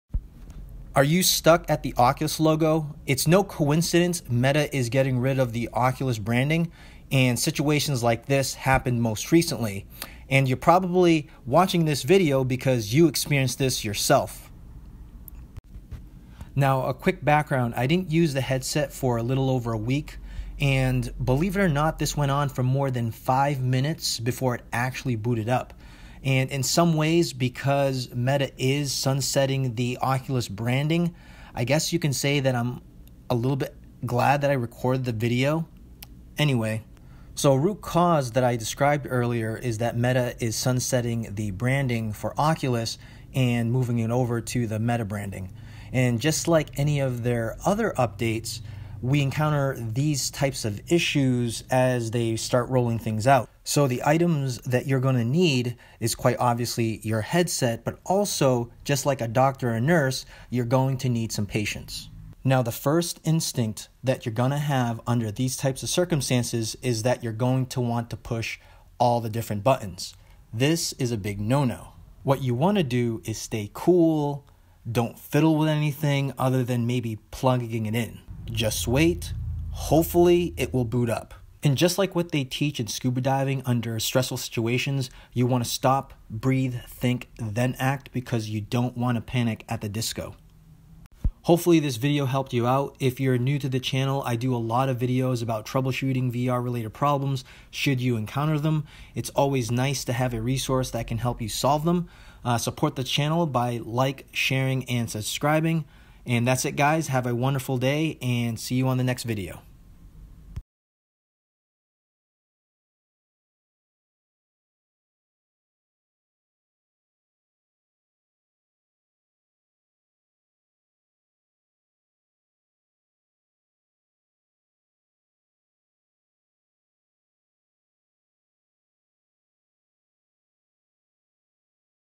Are you stuck at the Oculus Logo when starting up your Quest 2?
If you are stuck at the logo when booting up your headset, you are not alone. I experienced this most recently. Here is a simple way to resolve the issue.

0:00 Intro
0:15 Background
0:37 Root Cause
0:55 Item & Solution